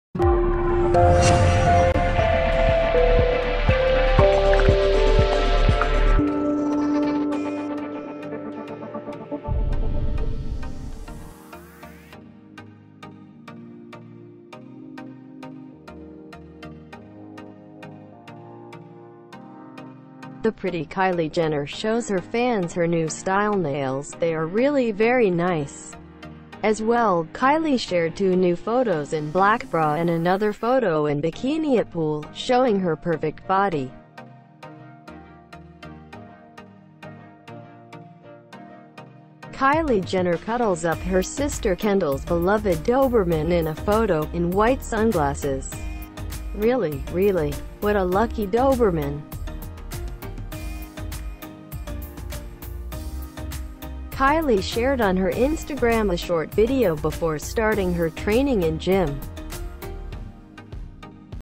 Kylie Jenner cuddles up to sister Kendall's beloved Doberman.
the pretty Kylie Jenner shows her fans her new style nails, they are really very nice!! as well, Kylie shared two new photos in black bra and another photo in Bikini at pool, showing her perfect body. As Kylie shared on her Instagram a short video before starting her training in Jim.